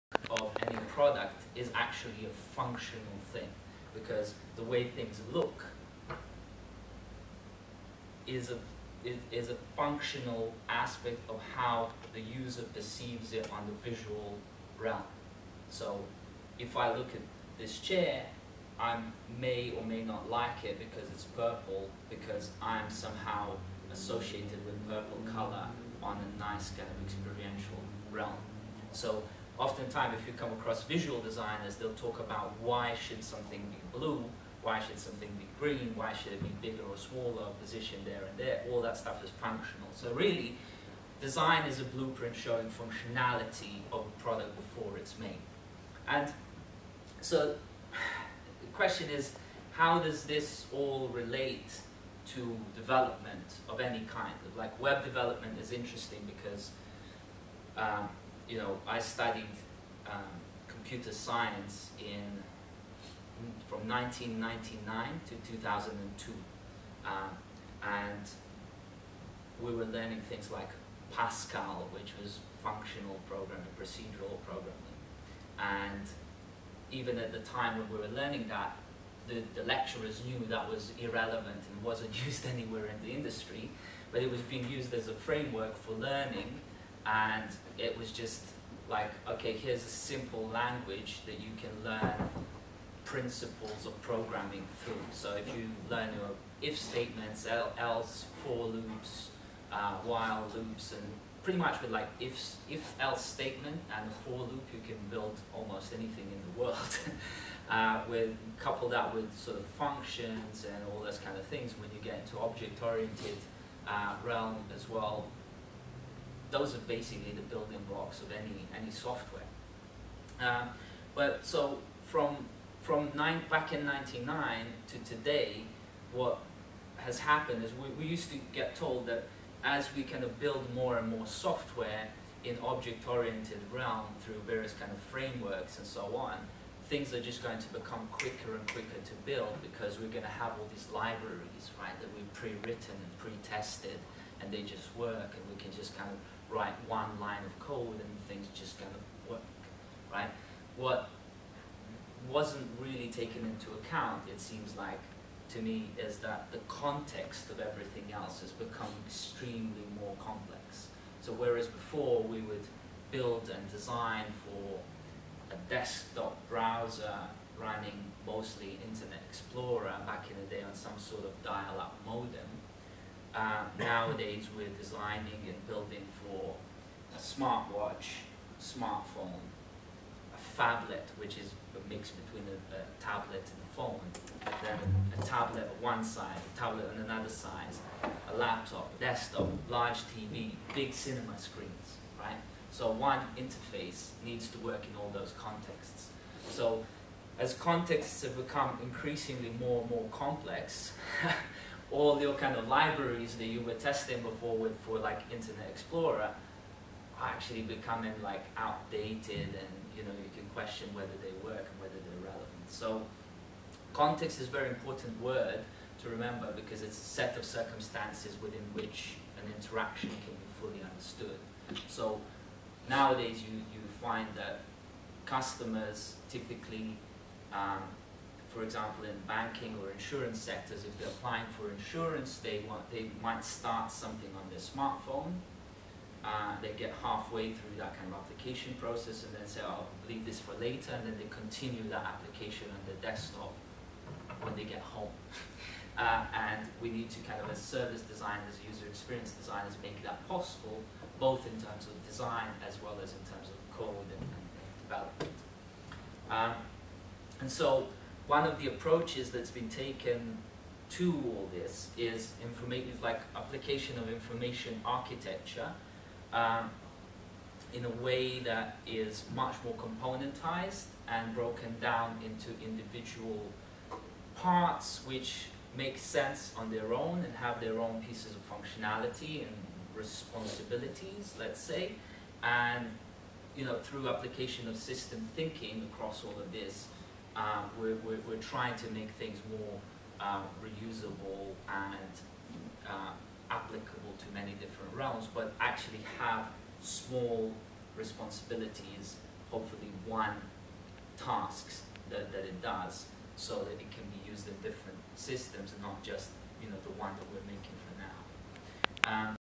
Bournemouth University Guest Lecture Sample
Jason Grant spoke to the 2nd year students of User Centered Web Development Degree course at Bournemouth University.